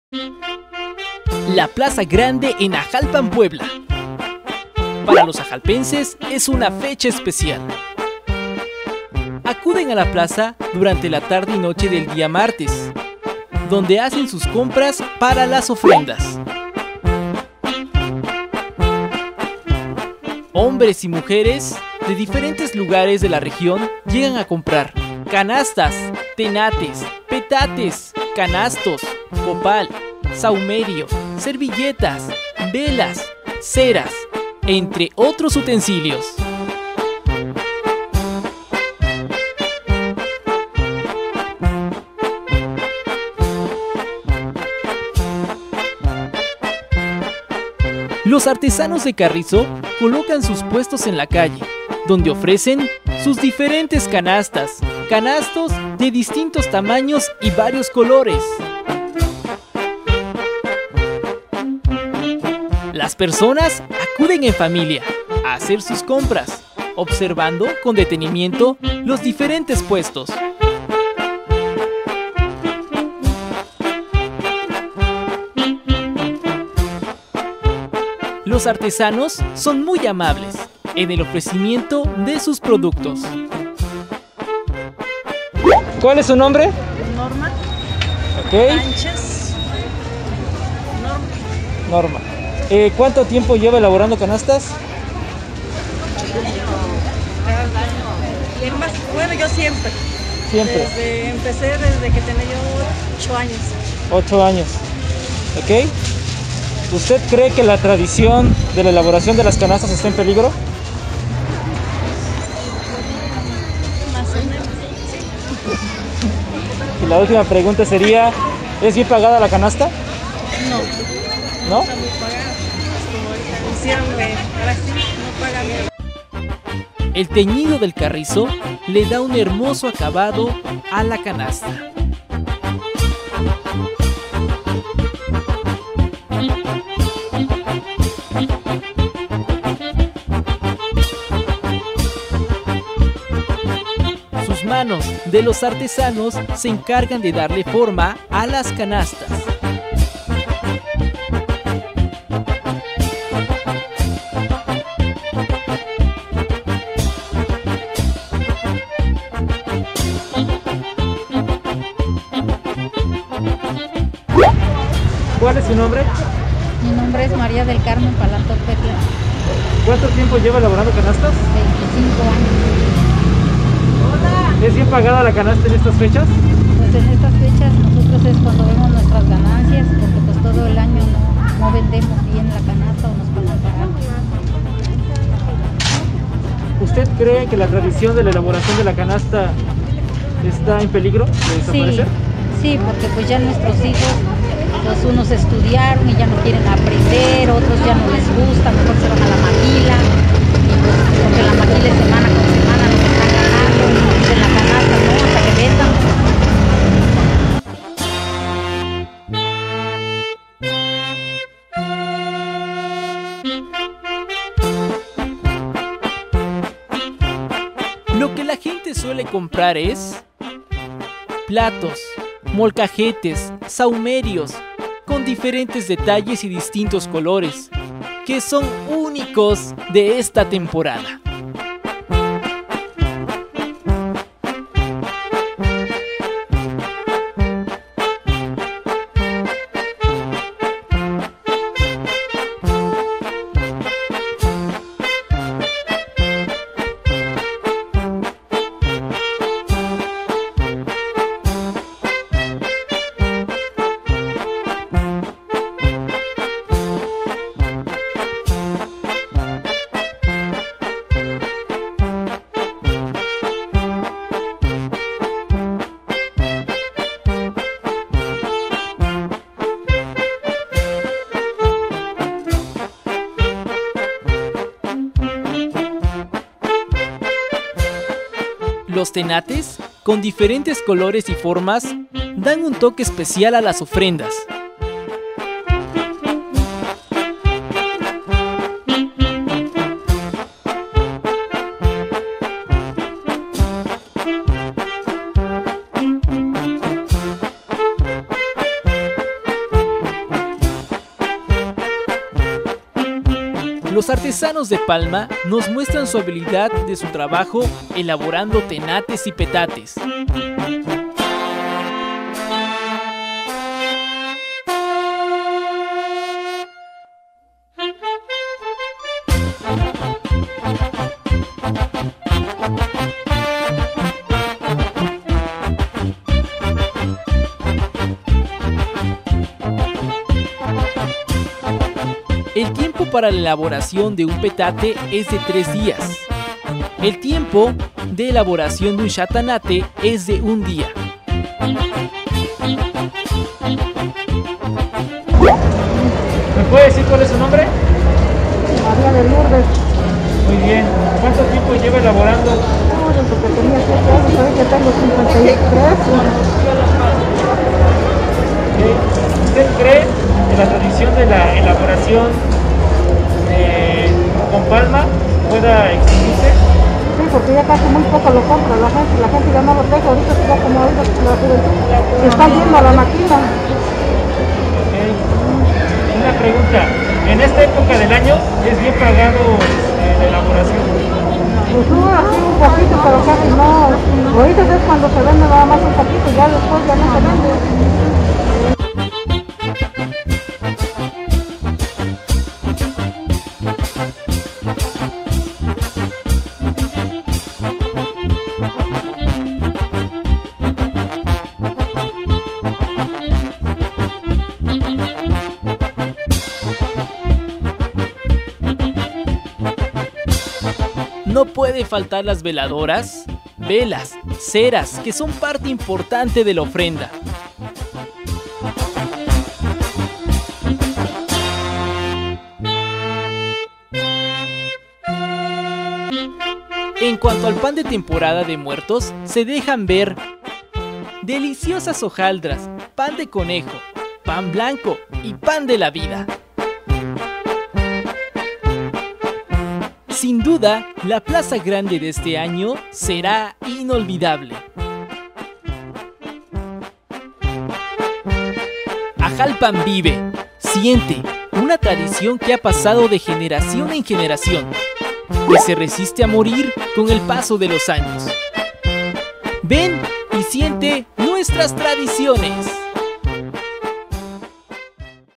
Tradiciones del día de muertos Ajalpan, Puebla, Plaza Grande. Parte 2
Este es el segundo video de varios que sacaremos por motivo del día de muertos, el cual tiene el fin de representar la tradicción de Ajalpan, Puebla, México. Espero que lo disfruten mucho y sigamos recordando a nuestros seres queridos que se nos adelantaron.